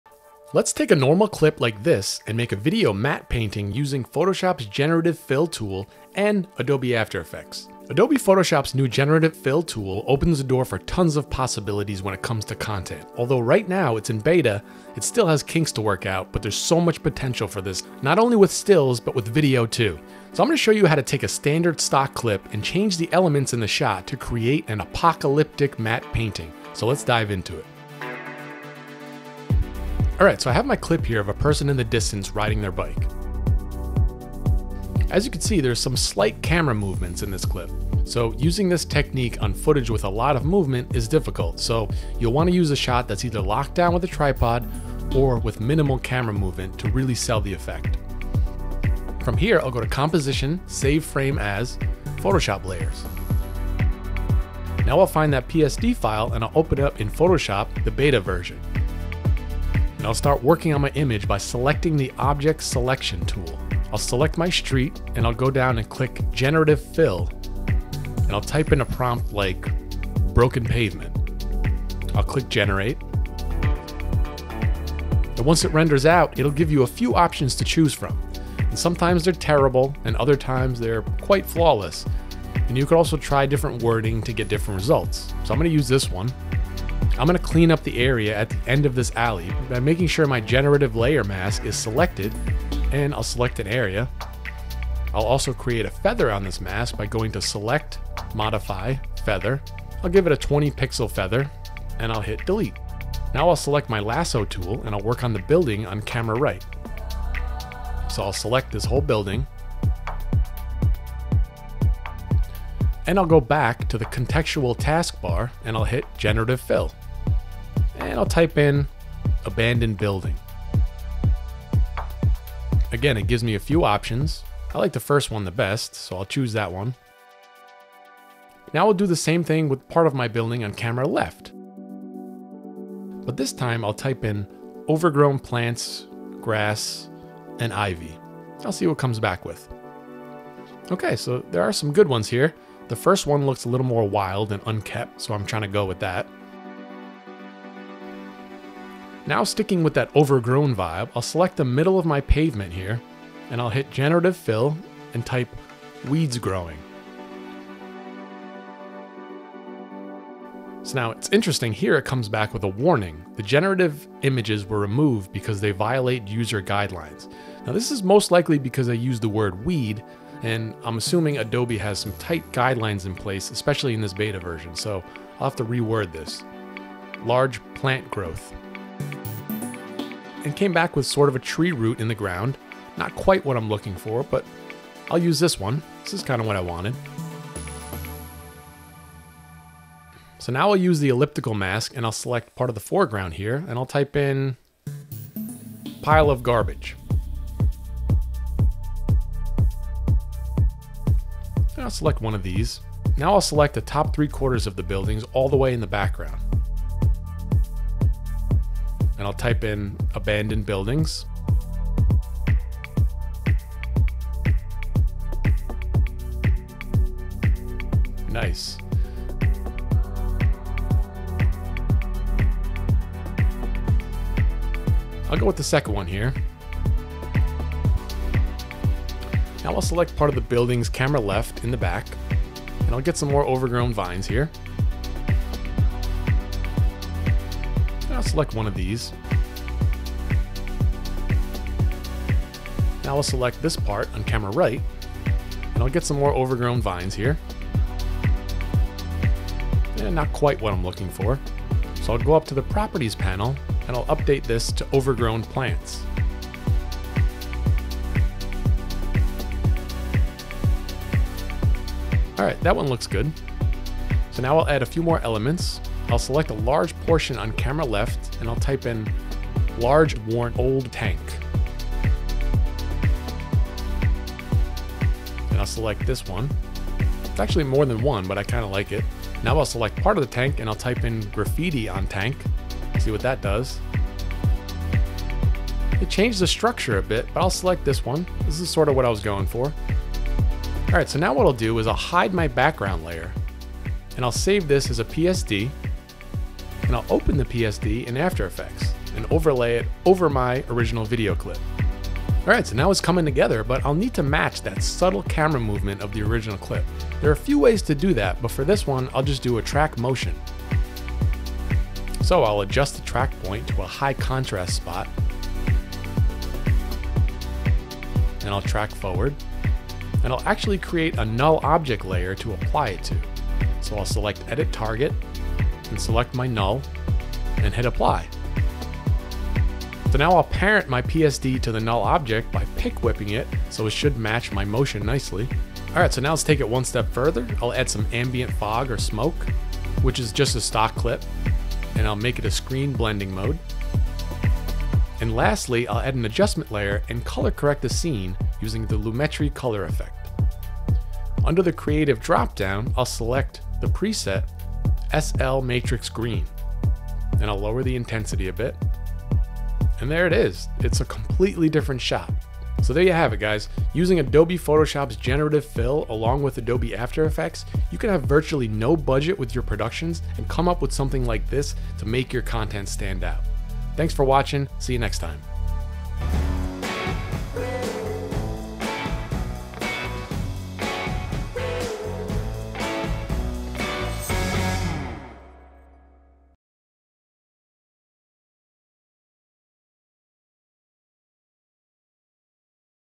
Transforming Videos with Generative Fill: Apocalyptic Matte Painting in Photoshop & After Effects
🤖 🎥  MY FAVORITE AI VIDEO TOOL

In this tutorial, learn how to transform a standard stock clip into an impressive apocalyptic matte painting using Adobe Photoshop's Generative AI tool and Adobe After Effects.

Discover the potential of this new tool and see how it can be used not only for still images but also for video. Follow along as we dive into the process of selecting elements, generating fills, and enhancing the scene. With these techniques, you can elevate your content and make it truly stand out without breaking the bank!